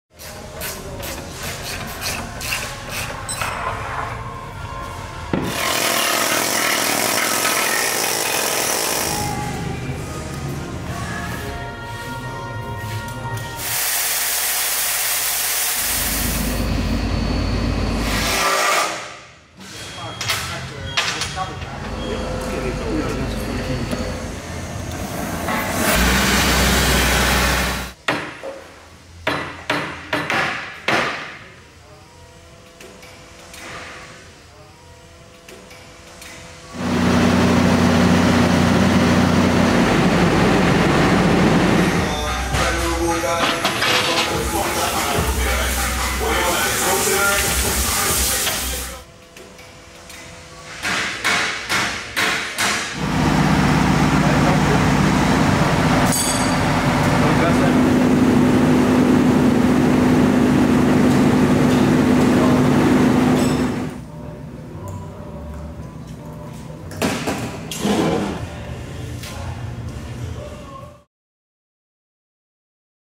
VÖGELE SUPER 1800-2 SPRAYJET repair - ALTCON Equipment
Our experts at our workshop in Haps working to get a VÖGELE SUPER 1800-2 SPRAYJET back on the road! 🔧🔩